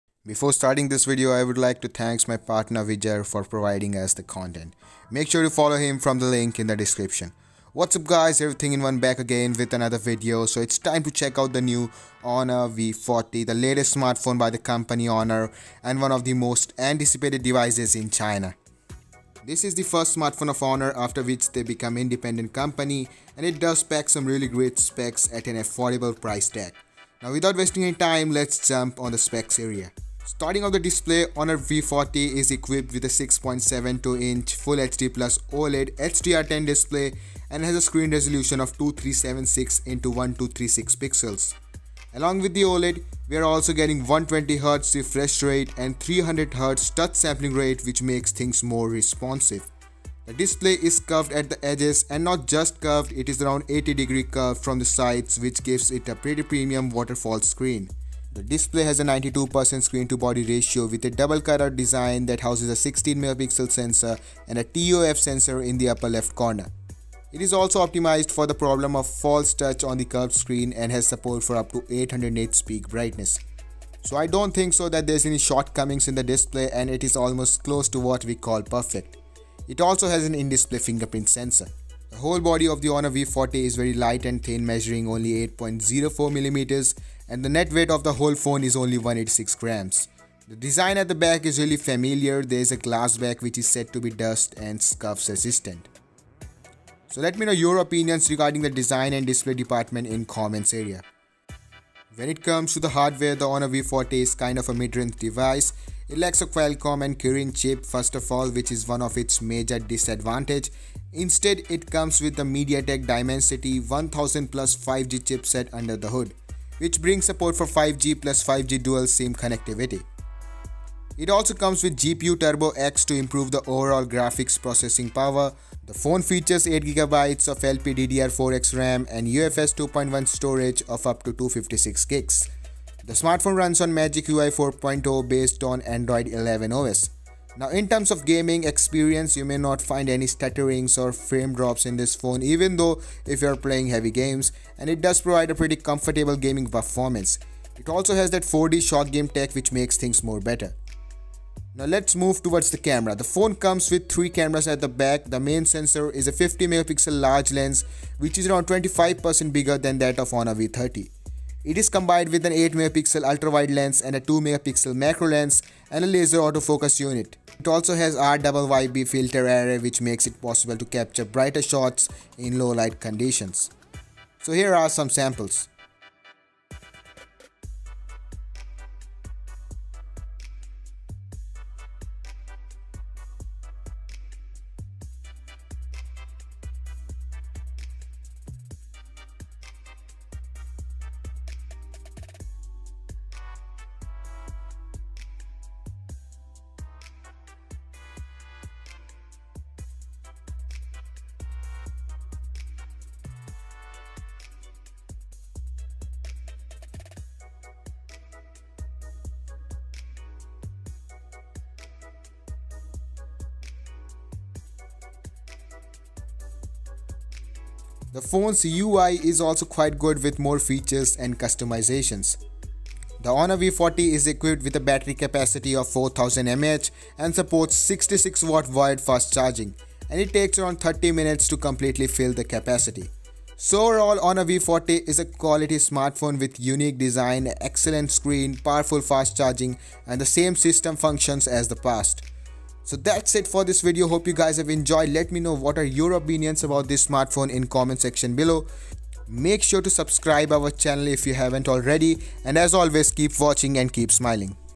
HONOR V40 Hands-on & Impression! Overpriced?
This Video belongs to our Partner & We have Full rights to use this video. NOT FOR REUSE!

8 GB RAM + 128 GB storage – 3,599 Yuan (~$556)

8 GB RAM + 256 GB storage – 3,999 Yuan (~$618)